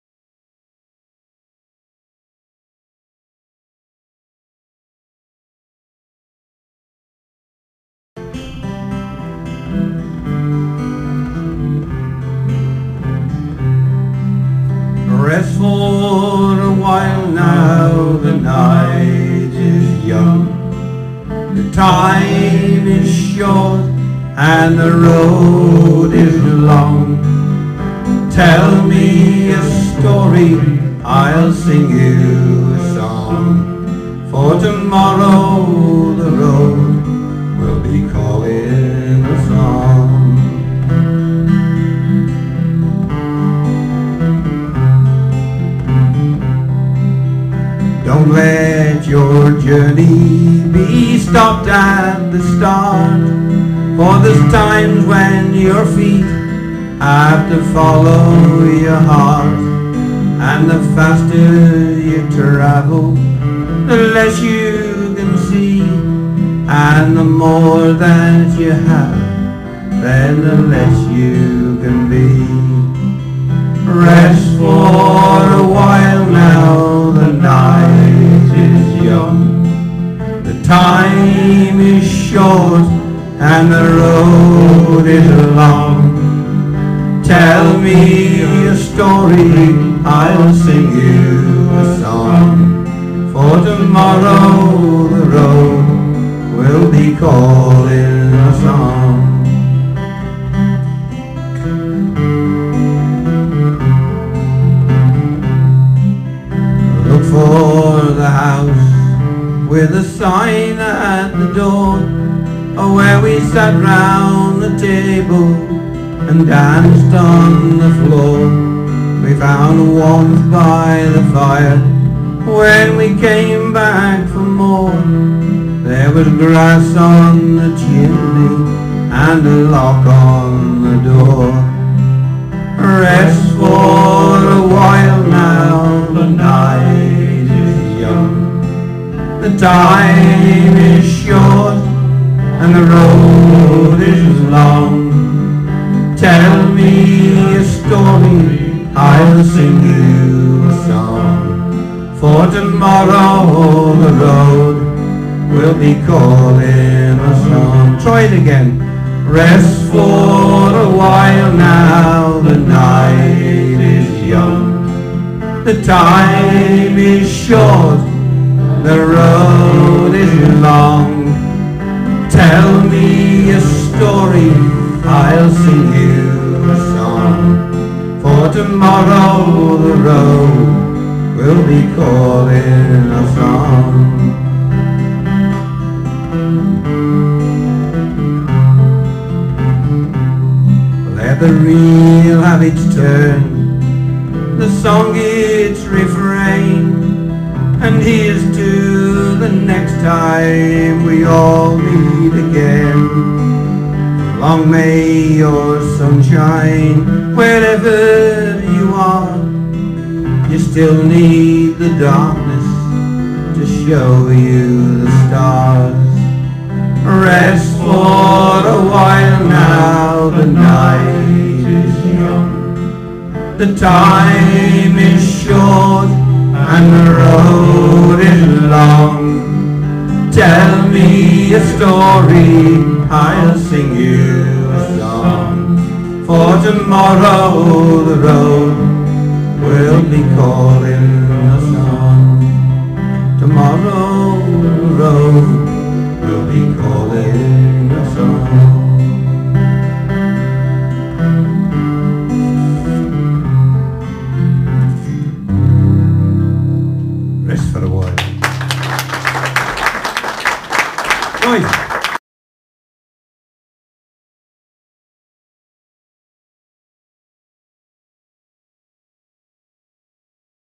The Staffordshire Men.wmv
Live in The Bridge Cross Club. Chase Terrace. Burntwood.